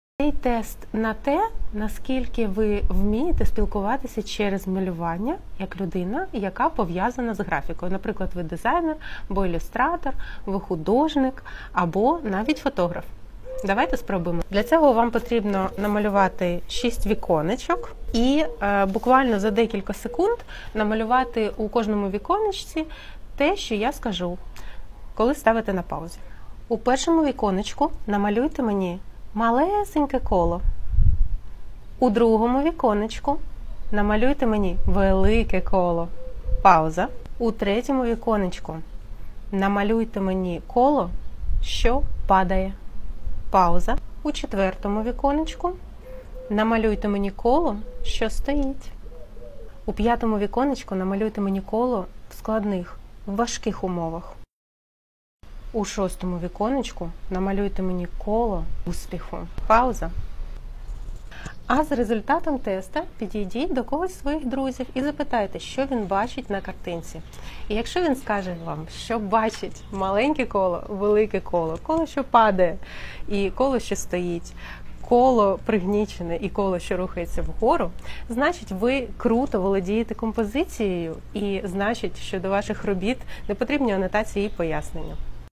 ТЕСТ НА КОМПОЗИЦІЮ | На скільки ви вмієте спілкуватися через малювання?
Якщо Ви пов'язані з графікою або маєте творчу професію, то цей тест для вас (художник, дизайнер, ілюстратор, фотограф). Після тесту краще питати декількох людей і використовуйте допоміжні питання: який? що робить? в якому стані?.